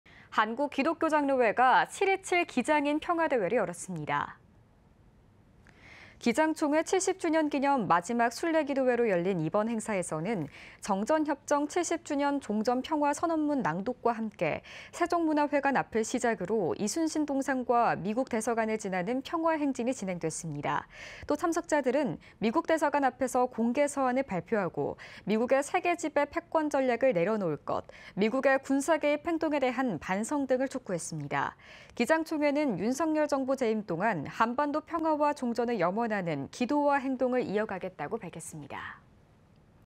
한국기독교장로회 7.27 기장인 평화대회 (이현지) l CTS뉴스
한국기독교장로회가 7.27. 기장인 평화대회를 열었습니다.

기장총회 70주년 기념 마지막 순례 기도회로 열린 이번 행사에서는 ‘정전협정 70주년 종전 평화 선언문’ 낭독과 함께 세종문화회관 앞을 시작으로 이순신 동상과 미국대사관을 지나는 평화행진이 진행됐습니다.

또, 참석자들은 미국 대사관 앞에서 공개서한을 발표하고, “미국의 세계지배 패권 전략을 내려놓을 것”, “미국의 군사개입 행동에 대한 반성” 등을 촉구했습니다.

기장 총회는 윤석열 정부 재임 동안 한반도 평화와 종전을 염원하는 기도와 행동을 이어가겠다고 밝혔습니다.